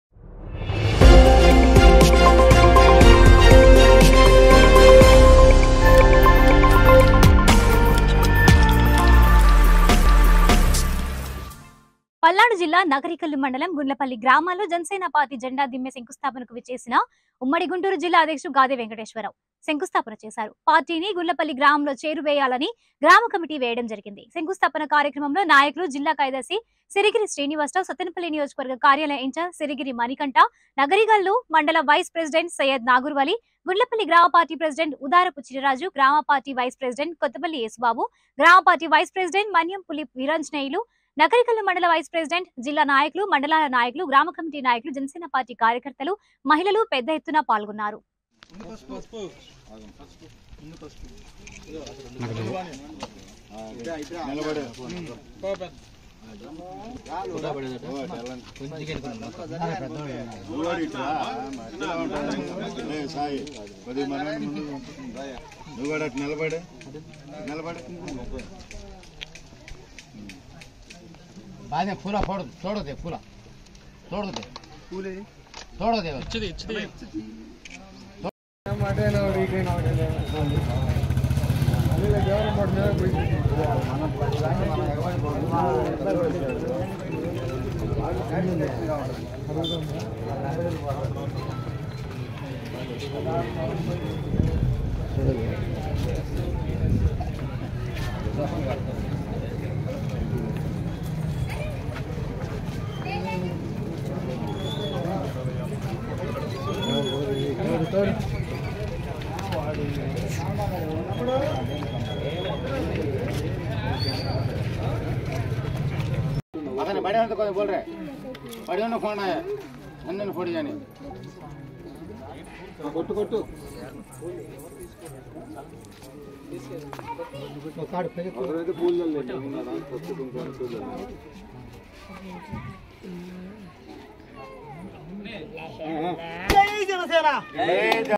గుండ్లపల్లి గ్రామాల్లో జనసేన పార్టీ జండా దిమ్మె శంకుస్థాపనకు విచ్చేసిన#mitv
గుండ్లపల్లి గ్రామాల్లో జనసేన పార్టీ జండా దిమ్మె శంకుస్థాపనకు విచ్చేసిన